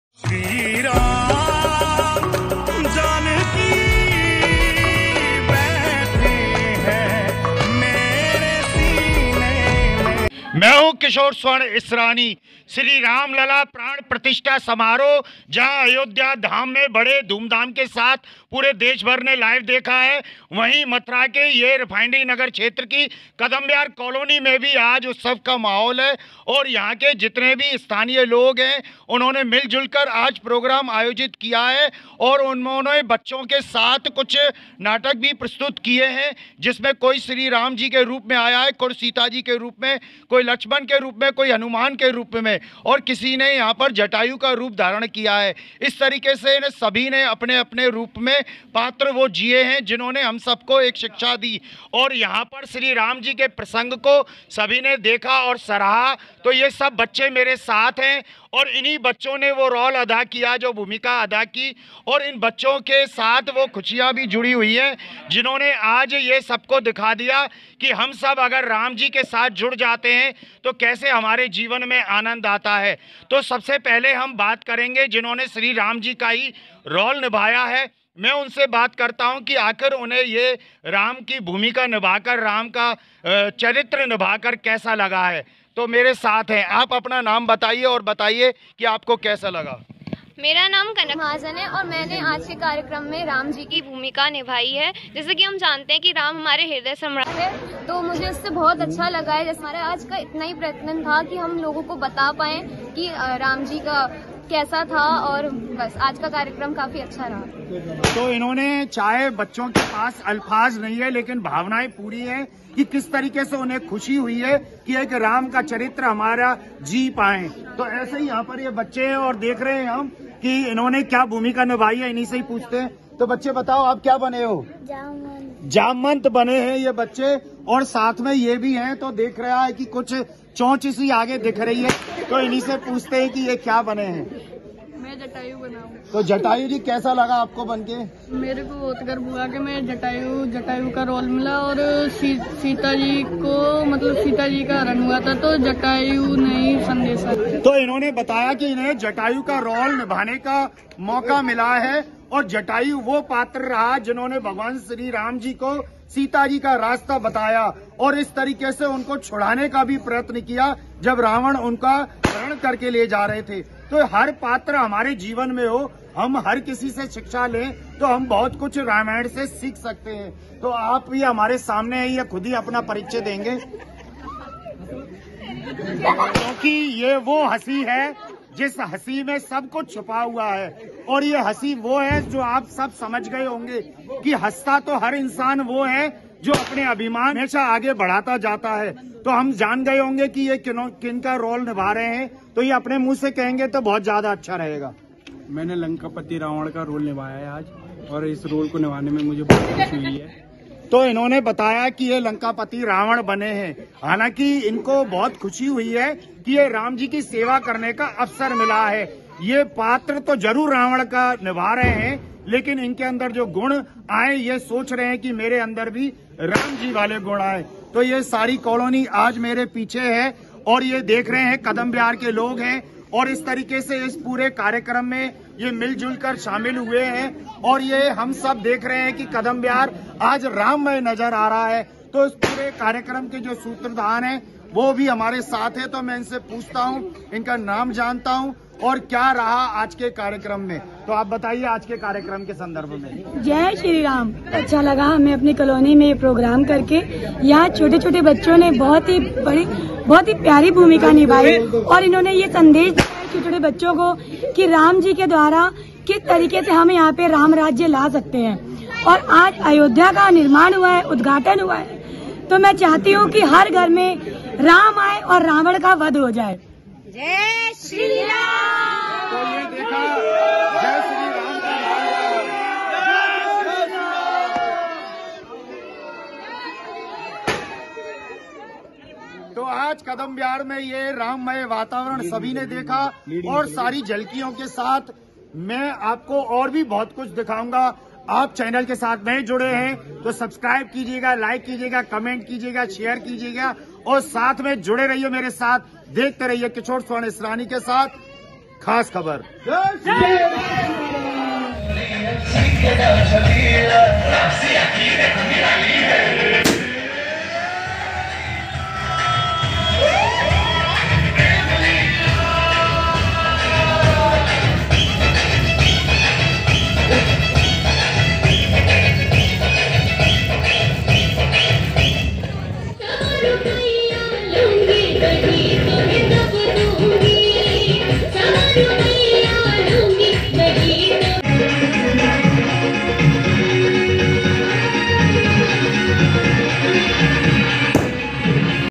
श्रीराम लला प्राण प्रतिष्ठा पर श्रीकृष्ण की नगरी में गली गली रामनाम से गूंजी | रामायण साकार हुई
श्रीराम लला प्राण प्रतिष्ठा के साथ ही श्रीकृष्ण नगरी हुई राममय, कदम्ब विहार में हुए विविध कार्यक्रम, हुआ सुंदरकांड का पाठ, सिंधी कालोनी में राममय वातावरण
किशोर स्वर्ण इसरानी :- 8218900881